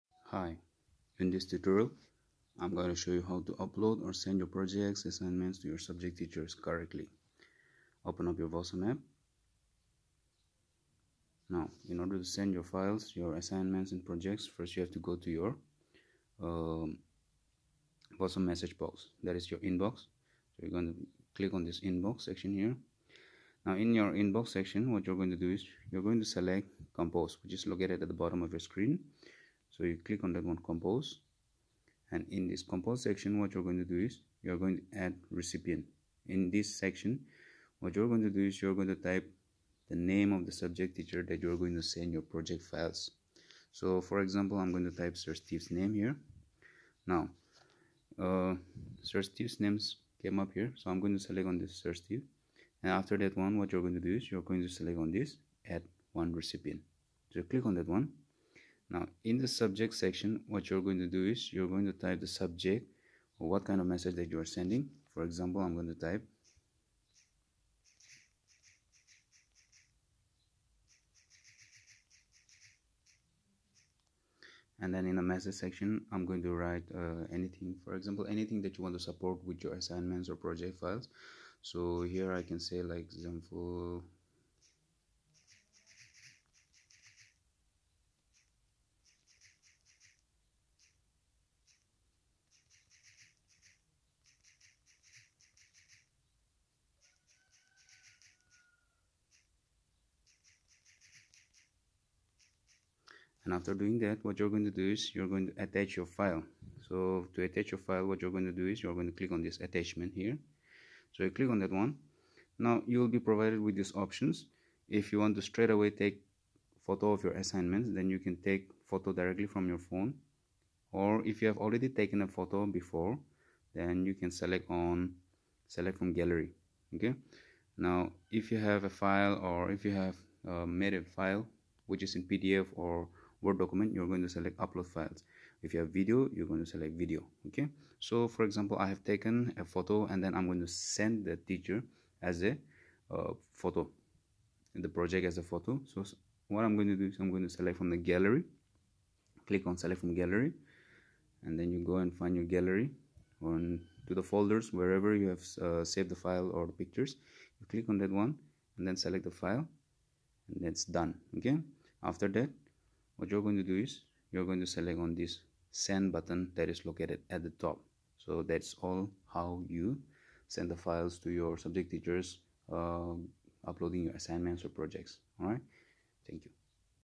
How to send files to your subject teacher-Vawsum App
In this video tutorial, I explain through the process of how to send file/photos/attachments to the subject teacher directly. I made this video on request of the teachers as many parents/students were sending their files or messages to the admin instead of the teacher. I hope that this video will help you.